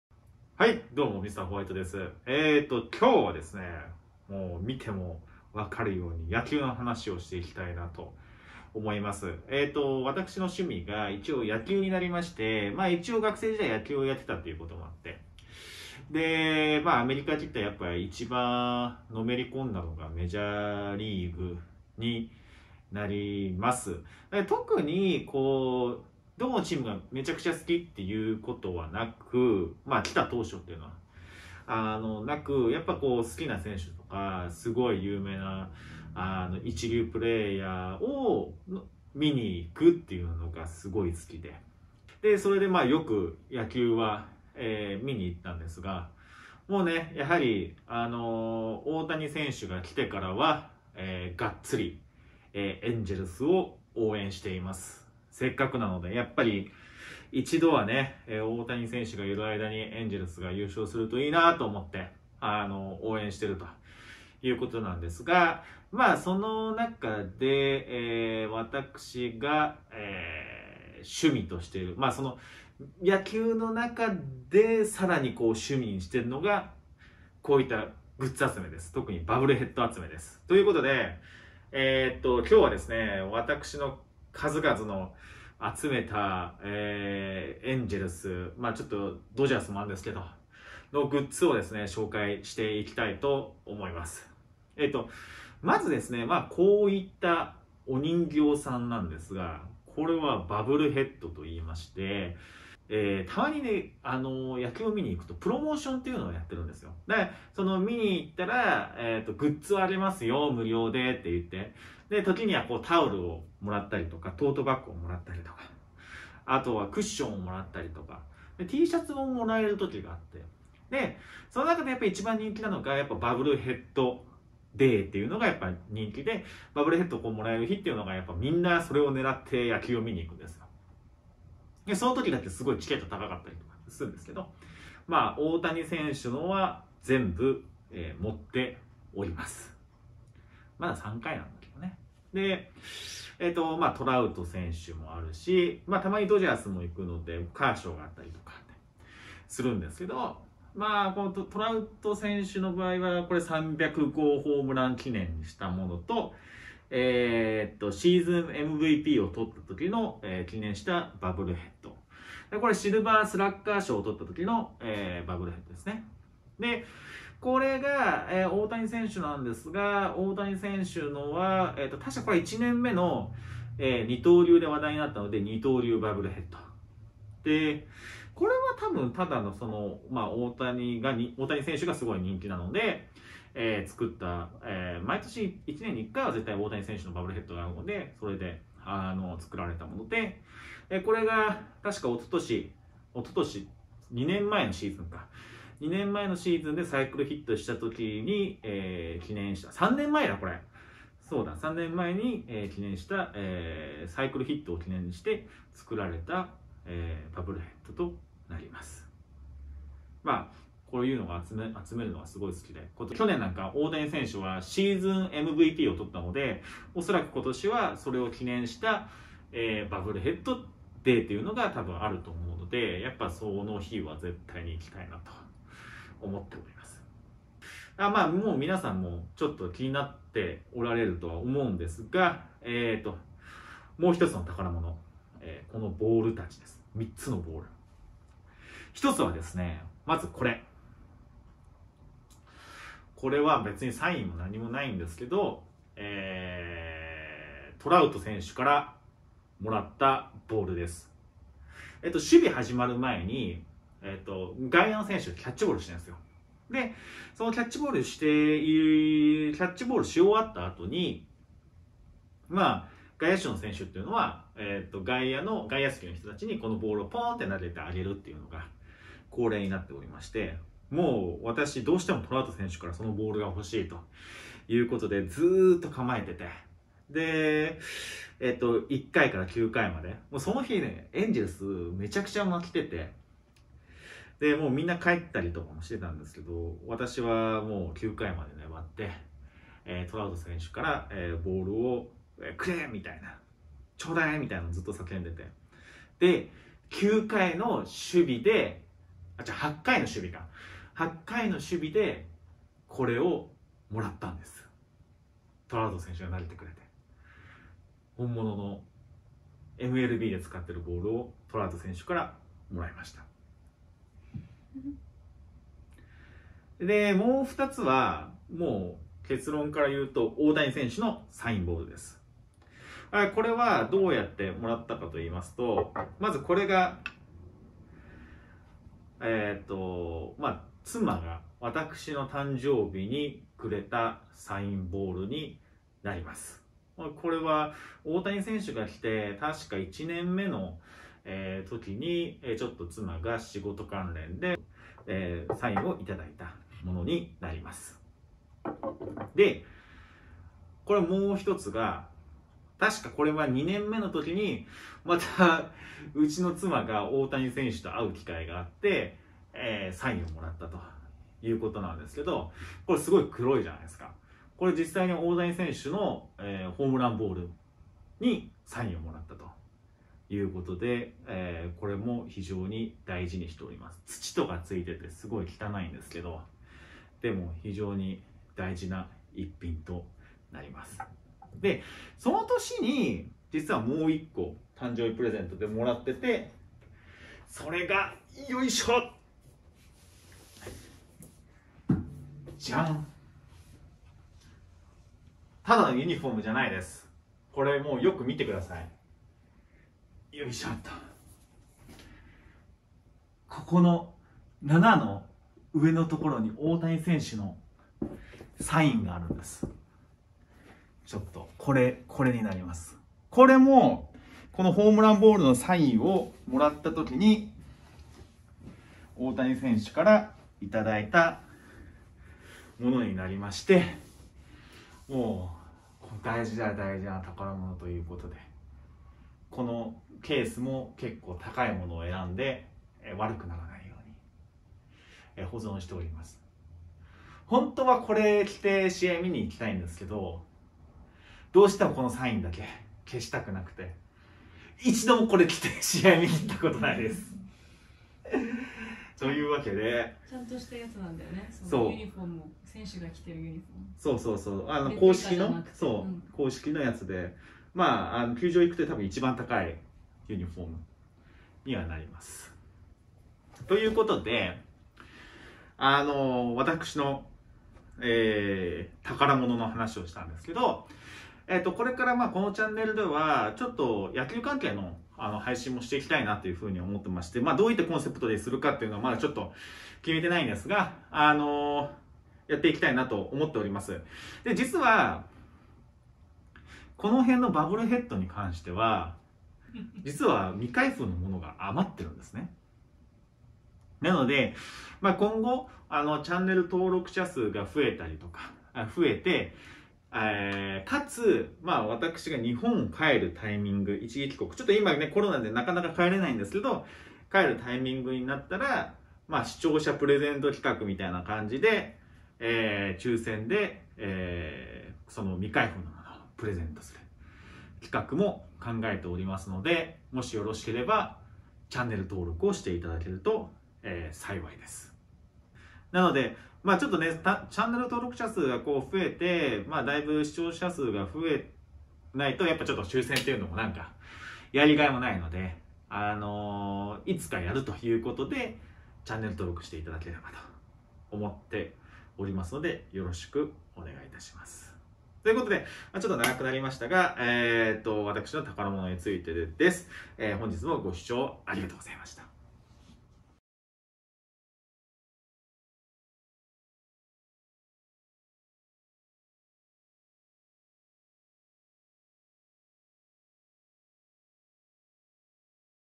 宝物を大公開！！大谷/トラウト選手のバブルヘッドコレクションからサインボールなどなど～
私の宝物を大公開！
バブルヘッドから大谷のサインボールまで！
バブルヘッドは未開封のものが余っているので、区切りの良いチャンネル登録者数達成したときにでも視聴者プレゼントをしようと思うので、是非チャンネル登録よろしくお願いいたします。